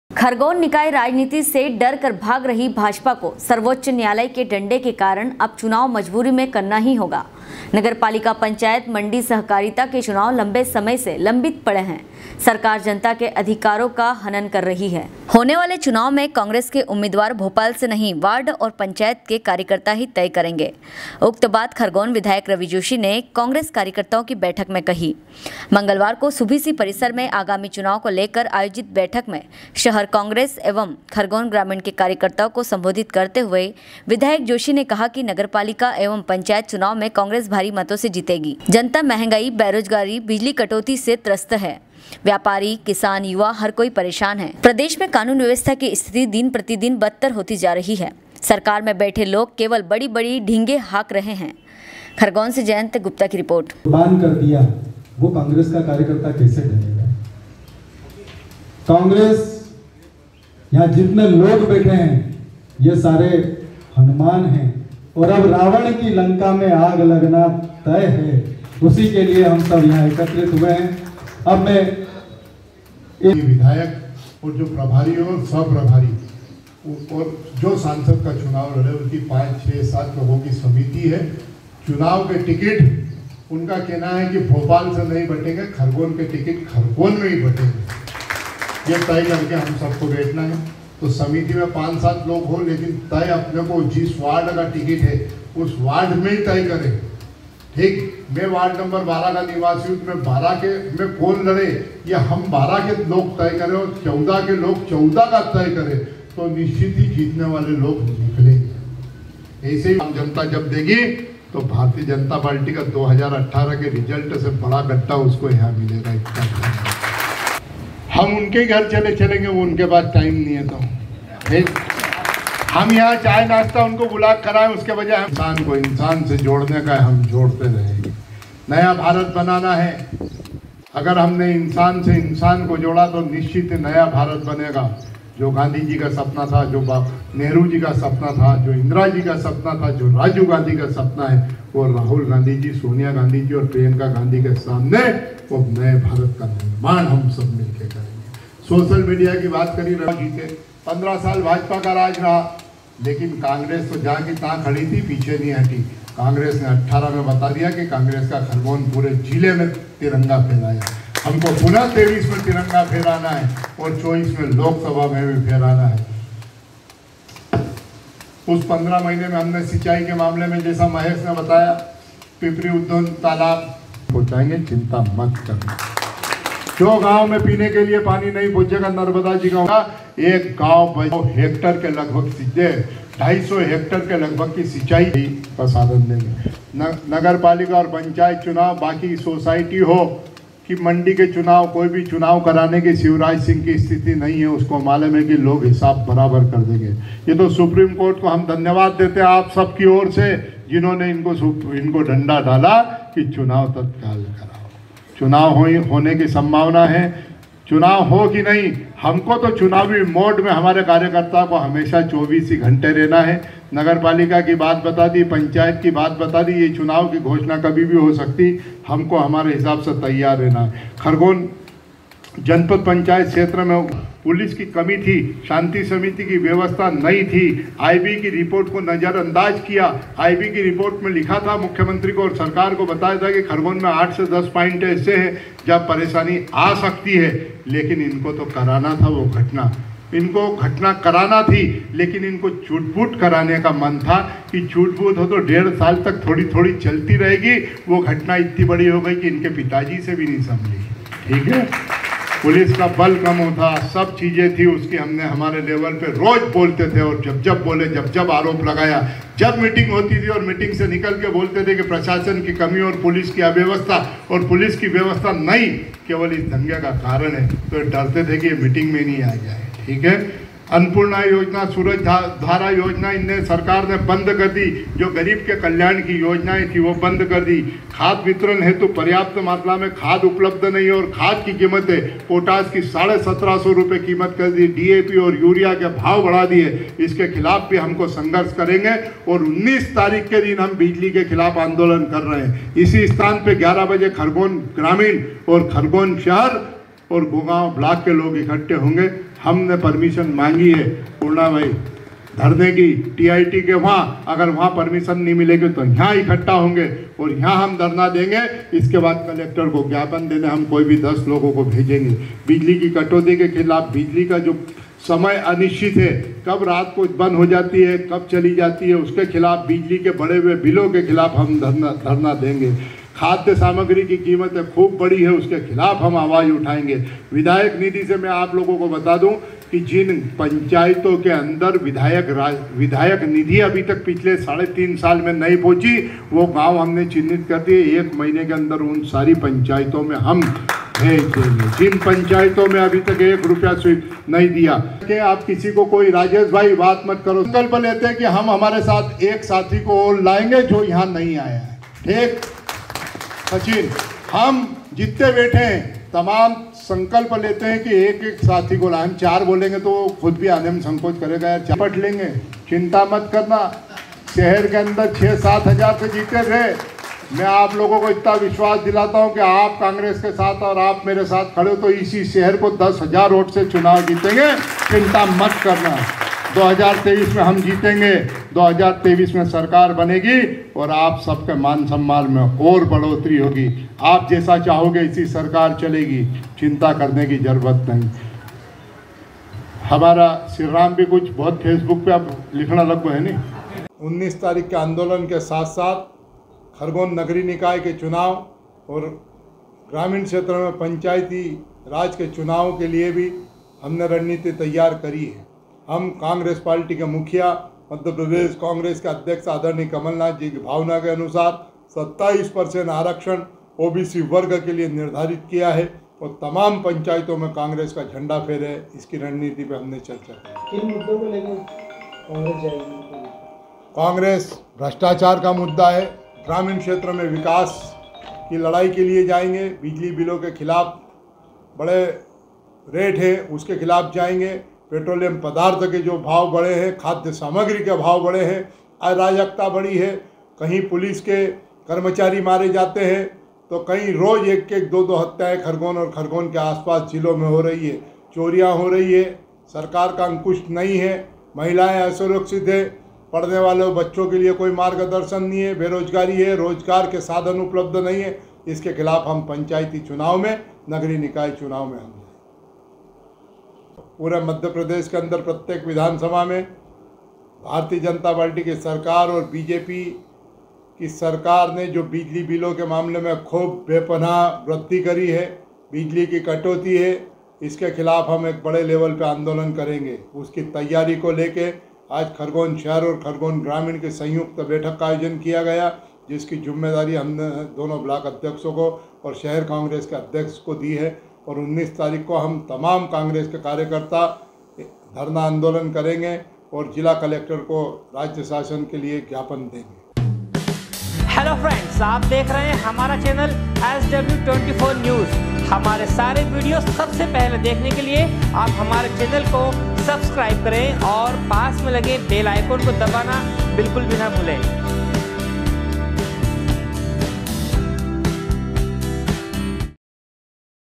भाजपा को सर्वाेच्च न्यायालय के डंडे के कारण अब चुनाव मजबुरी में कराना ही होगा
खरगोन निकाय राजनीति से डर कर भाग रही भाजपा को सर्वाेच्च न्यायालय के डंडे के कारण अब चुनाव मजबुरी में कराना ही होगा। नगर पालिका, पंचायत, मंडी, सहकारिता के चुनाव लंबे समय से लंबित पड़े है। सरकार जनता के अधिकारों का हनन कर रही है। खरगोन से जयन्त गुप्ता की रिर्पोट